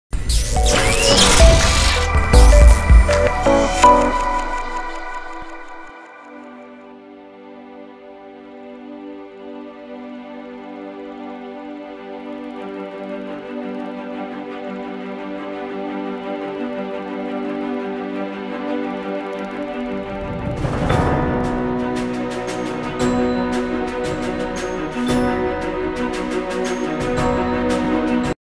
Tesla Grow Green Commercial
Using melodic vibration frequencies, and electrical stimulation we put vegetative life in harmony for maximum growth.